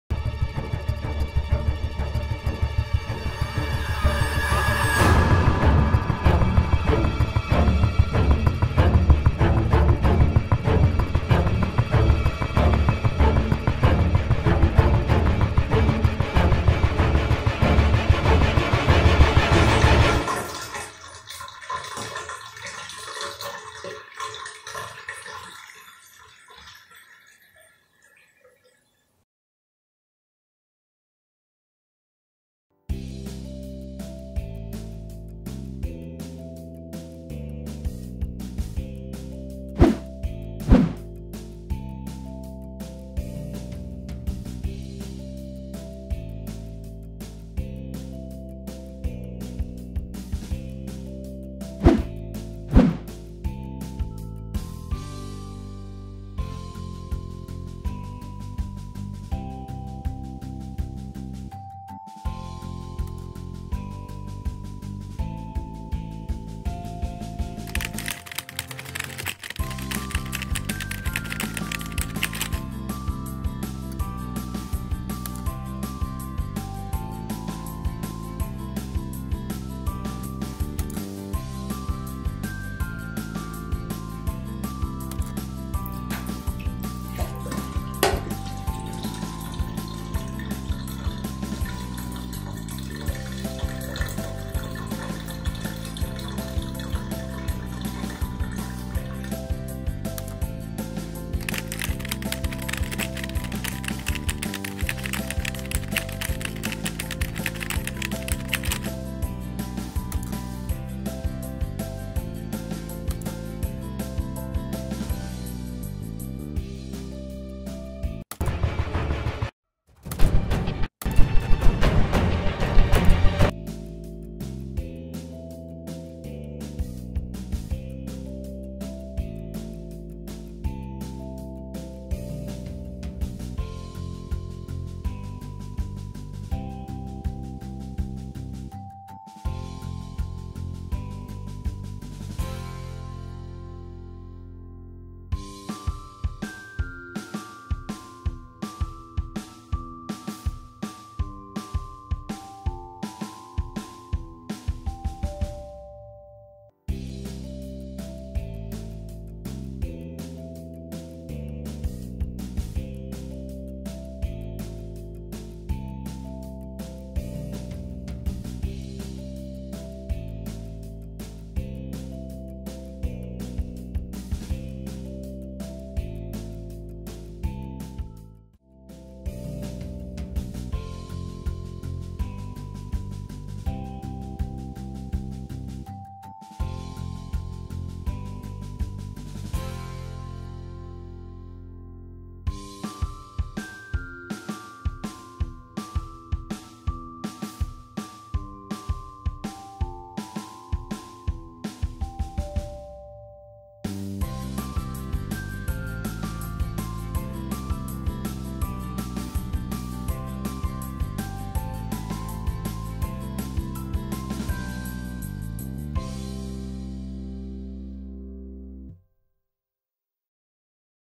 The Suspense Is Killing Me - Blender Water Simulation
I made a water simulation... then another one and another one... A year and a half later, I made this video putting the water in its own context. I wanted it to be as realistic as possible, but I only partially succeeded. I made the bathroom using real textures that I photographed and one that I found online, and I recorded real water sounds. Plus I used two-point lighting to make it seem more realistic. I even went so far as to make the cameras based on the Sigma 35mm f/1.4 lens. Needless to say, I had fun.